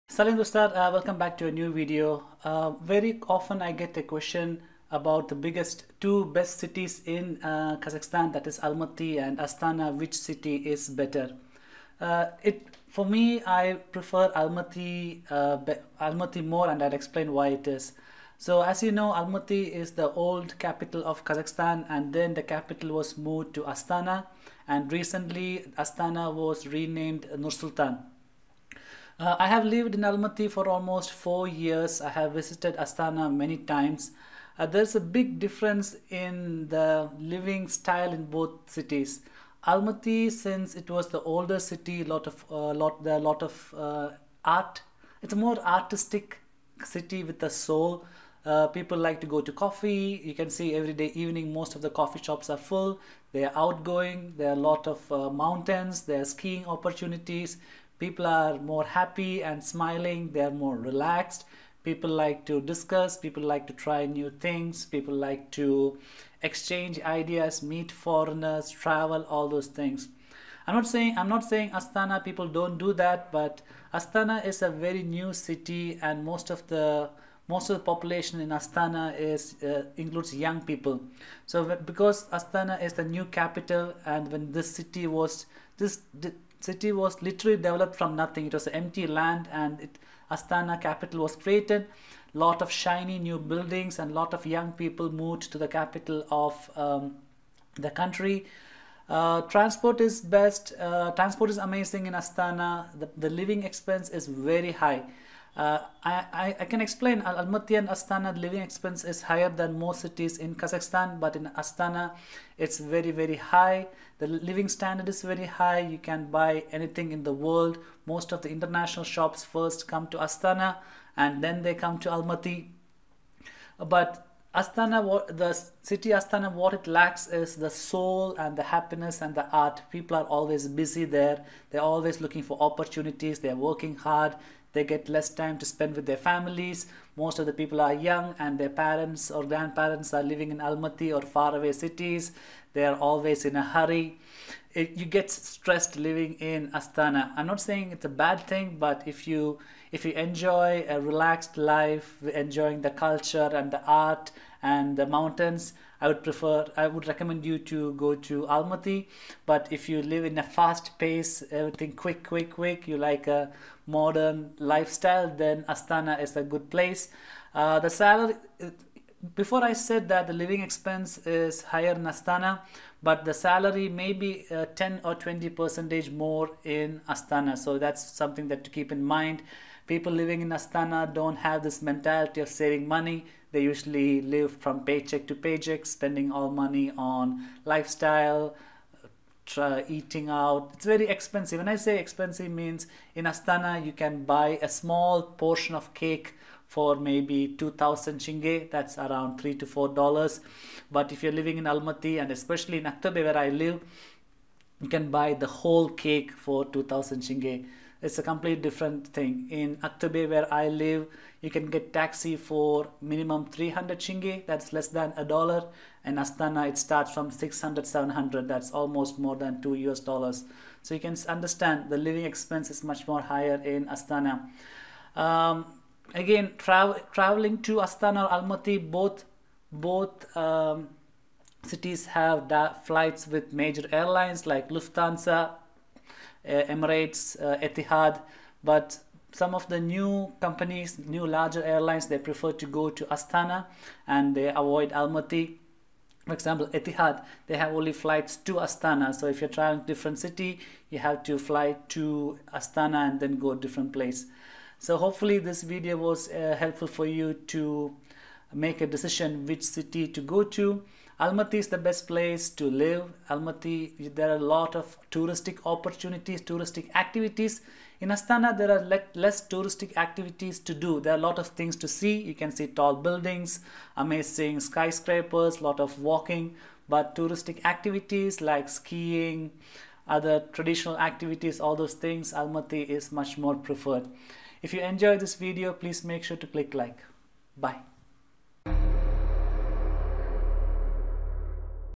Astana or Almaty ? Which is the best city?
You have much more options around Almaty and in Southern Kazakhstan. If you want to visit Astana, you could take the overnight train (17hrs) or a flight with Air Astana